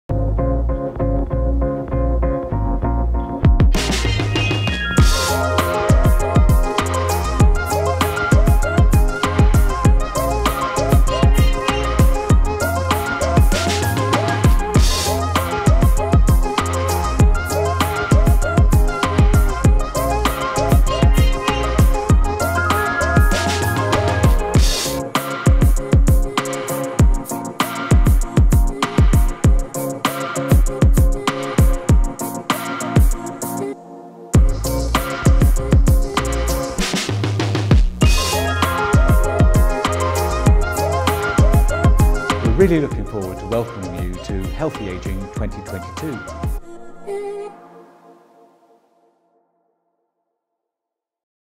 Healthy Ageing 2022 Conference Promo Video
Join Healthy Ageing 2022 to meet innovators, researchers, policy makers and investors working on projects and activities to support healthy ageing.